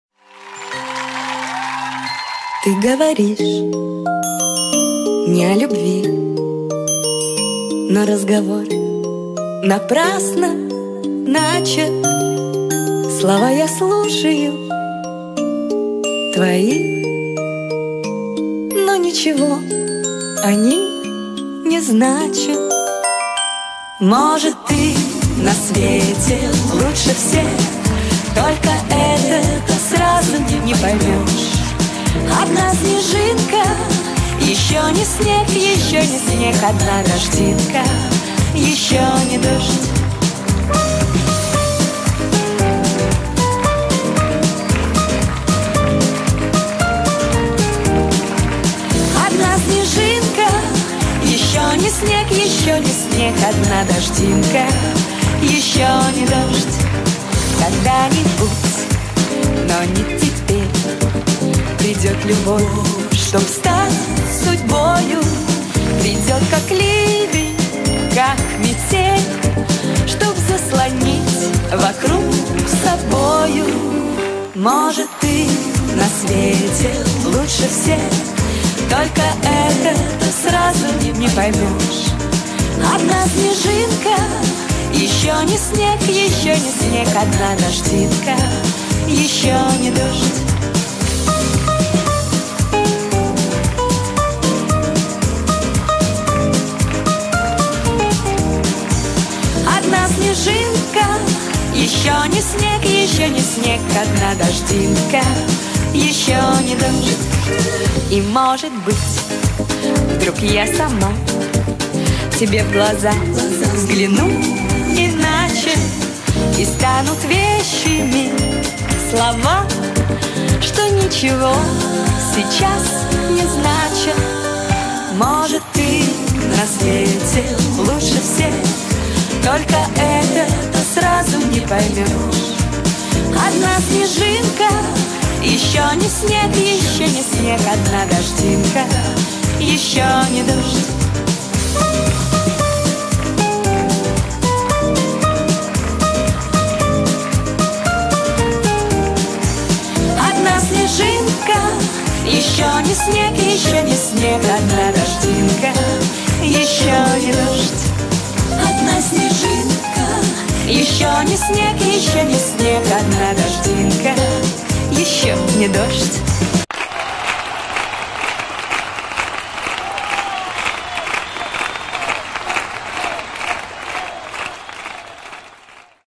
Zhanna Friske. Remember it so (Жанна Фриске. Запомните её такой)
Памяти актрисы, певицы, женщины-бойца и просто- народной любимицы.
Благодарю авторов стилей, переходов и футажей.